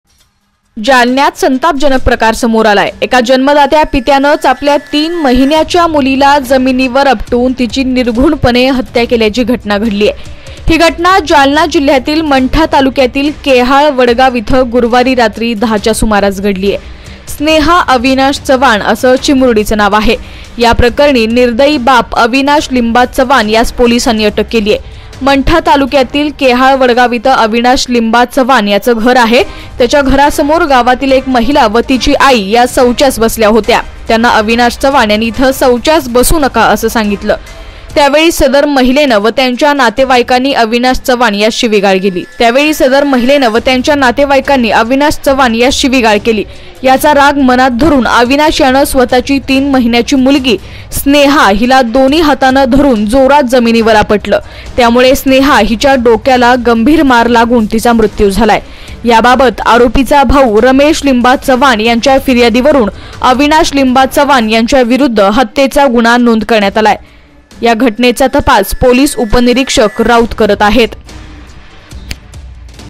बापाचं बनला कसाई । शेजाऱ्यावरील राग काढला तीन महिन्याच्या चिमुकलीवर, स्वतःच केला लेकीचा घात
शेजाऱ्यान वरील राग काढला तीन महिन्याच्या चिमुकलीवर ,बापाचं बनला कसाई स्वतःच केला लेकीचा घात